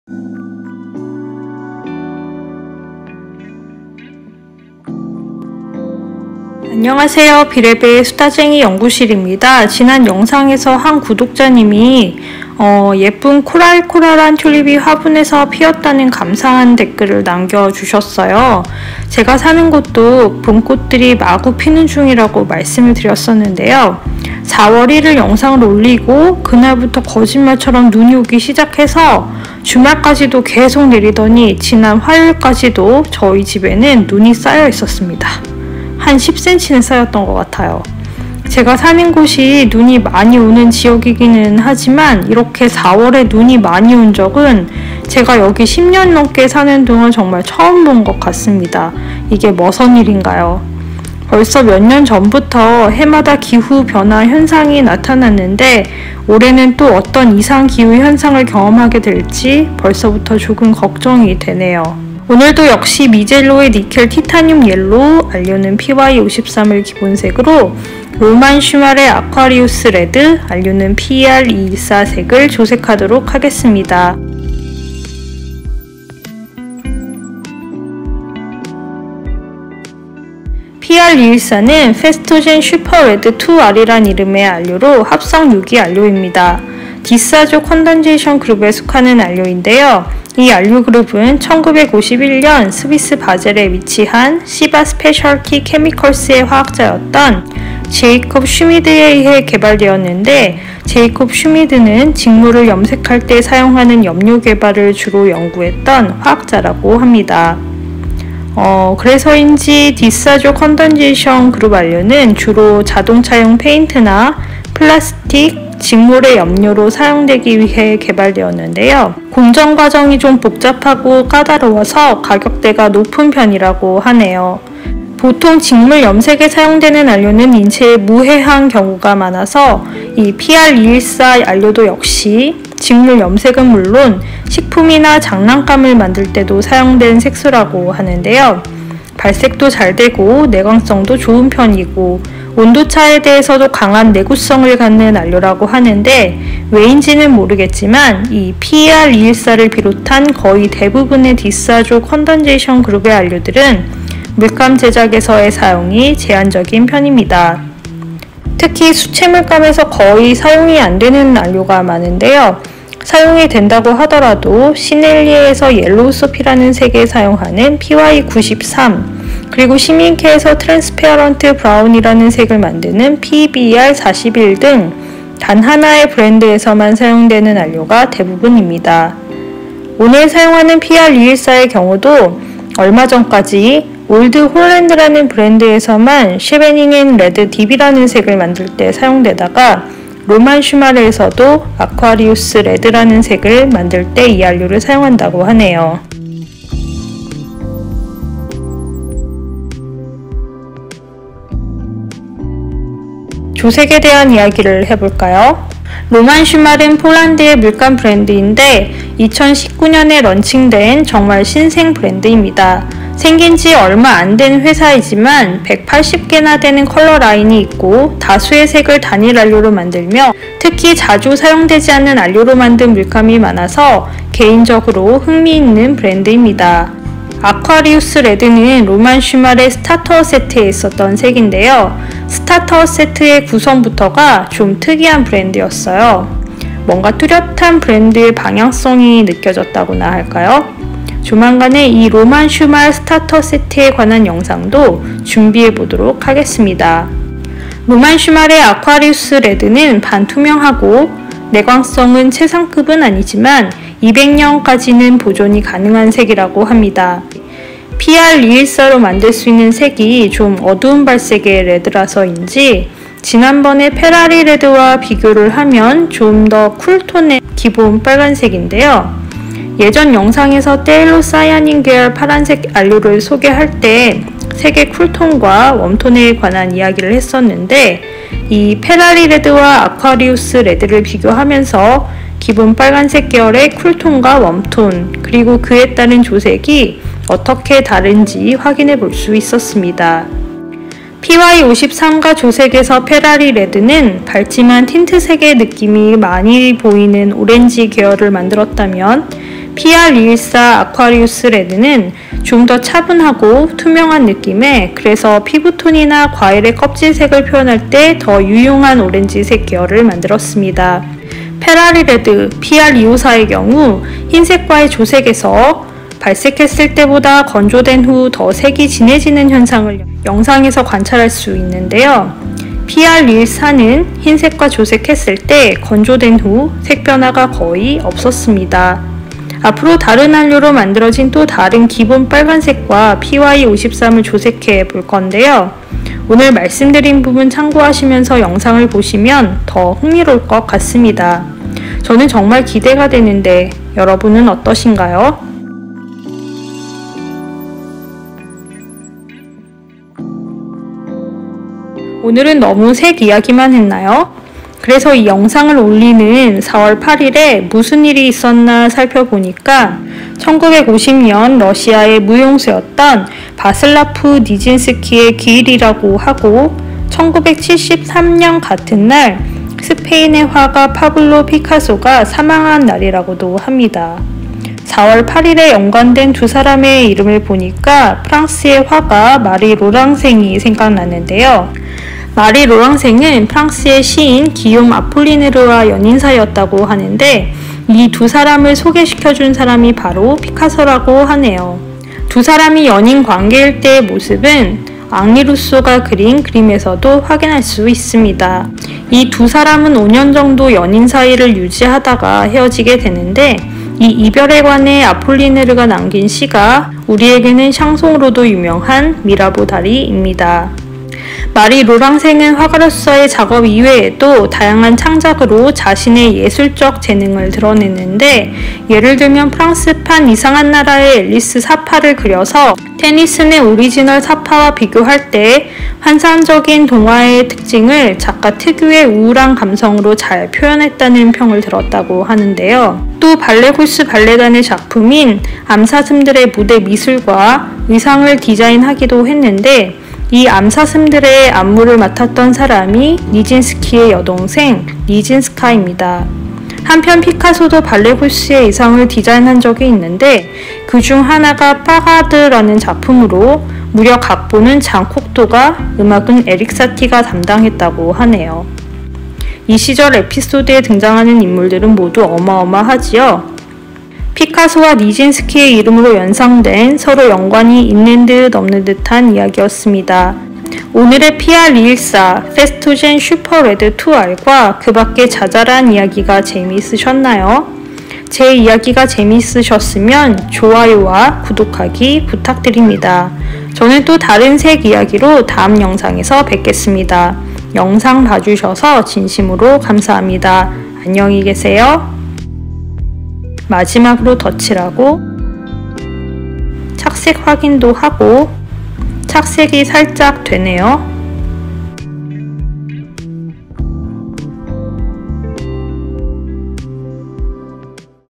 [수채물감 조색] 미젤로, Nickel Titanium Yellow_PY53+ 로만 슈말 Aquarius Red_PR214 (자막있음)
안녕하세요,

오늘은 Mijello의 Nickel Titanium Yellow(PY53)와 Roman Szmal의 Aquarius Red_PR214 조색 영상을 올립니다.

미젤로의 니켈 티타늄 옐로우 색이 궁금하신 분들은 다음 영상을 참고하세요.

Paper - Hahnemühle expression, cold press, coton 100%, 300g
Brush - Rhaphaël, softaqua 8056, n° 0
              Léonard, 1160PL ArtStyl’ n° 6
Paints - Mijello, Nickel Titanium Yellow_PY53
               Roman Szmal, Aquarius Red_PR214
               Mijello, mission design color, matte white_PW6
               Winsor and Newton, Ivory black_PBk7